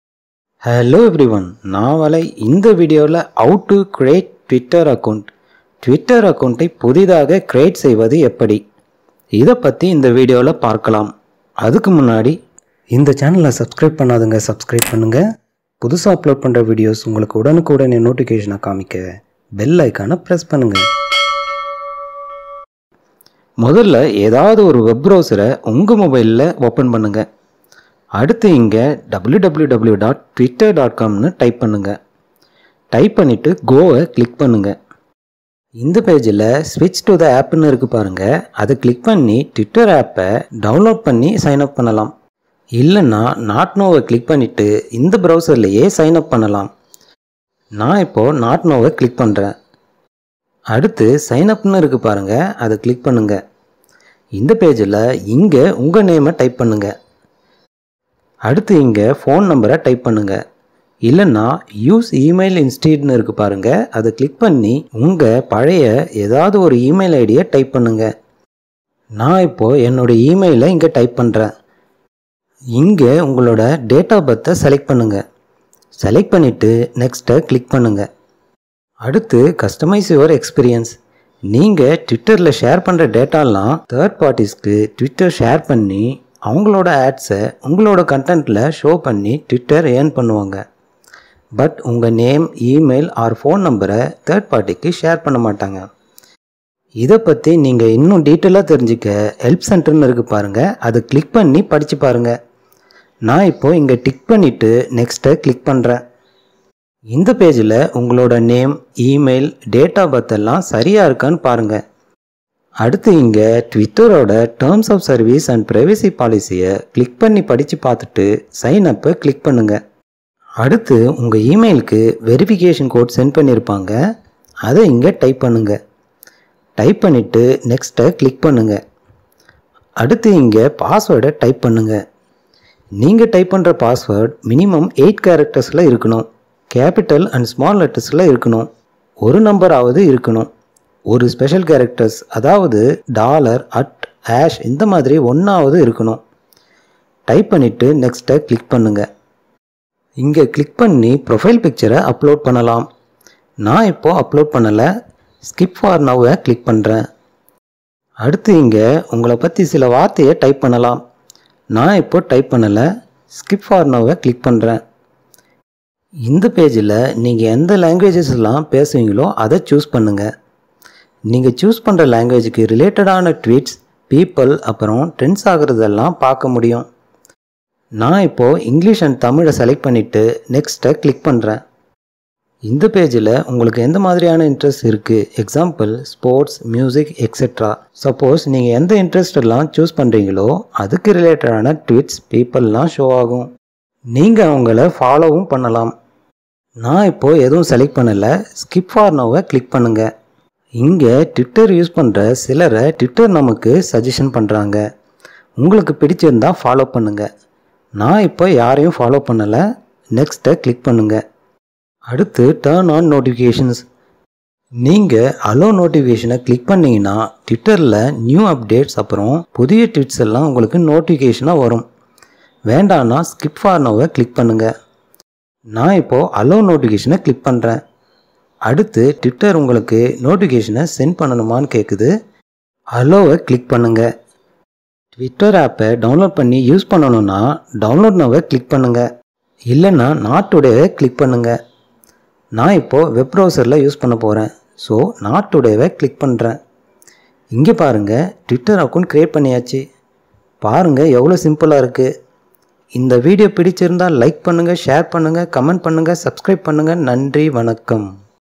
How to Create Twitter Account | In Tamil | Tamil Tech Channel
How to Create Twitter Account | Mobile | In Tamil | மொபைலில் சுலபமாக Twitter accountai create செய்வது எப்படி?? This video talks about How to Create Twitter Account with easy steps. It explains a simple method to understand everyone.